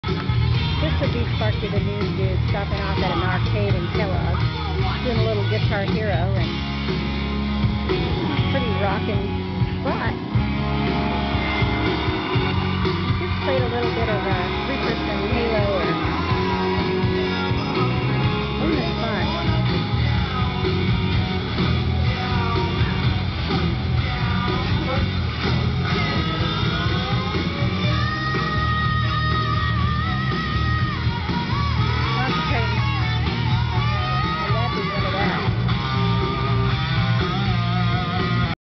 Sparky finds a gamer haven
In Kellogg Idaho.. these guys rock. A 3$per hour play games like Halo, GHero and alot more. They're really paintball experts with lots of supplies and extension of the sport.